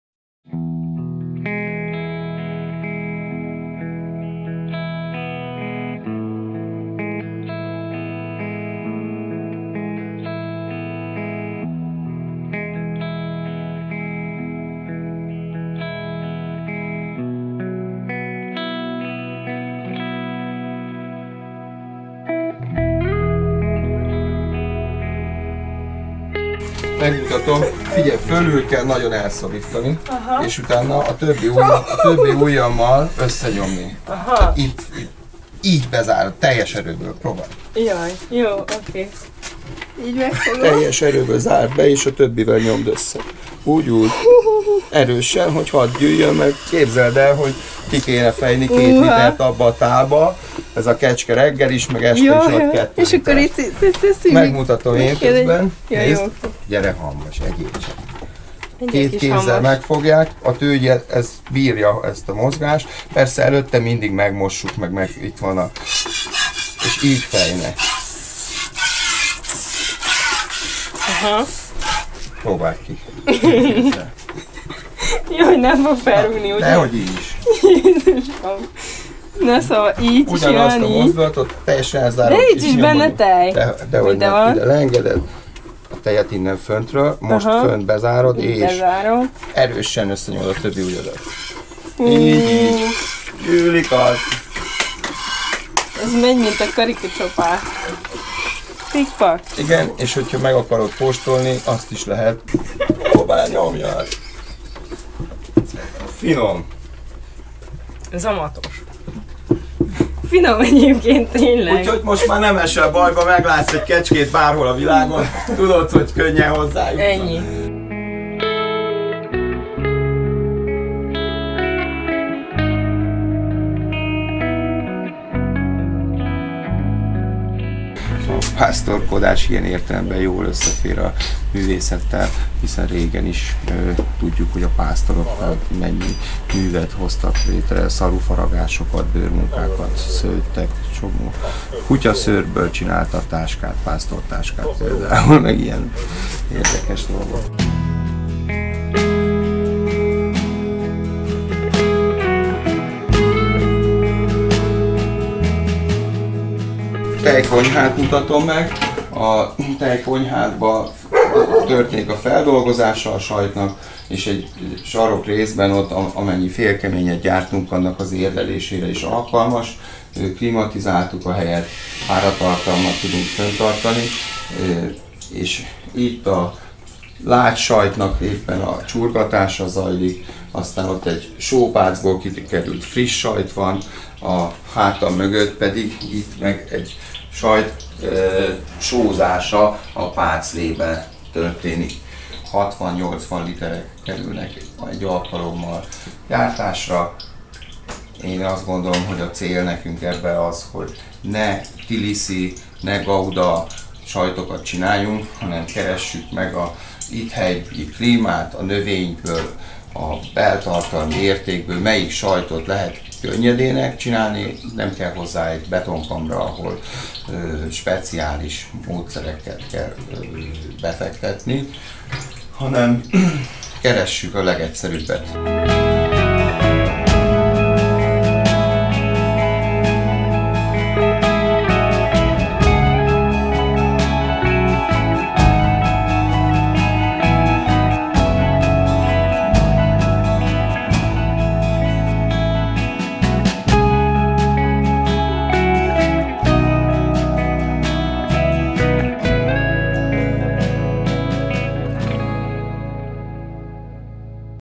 Valóságos nyugalom szigete a mecseki szobrász-kecskepásztor birtoka
Illa Gábor szobrász nem akarta maga mögött hagyni szakmáját és ez által művészetét, a szobrászatot. Kihasználva családi telkük előnyeit fogta magát, vett két kecskét és eldöntötte: kecskesajtot fog csinálni a szobrok mellett. Mi pedig utánajártunk, hogyan is történt mindez.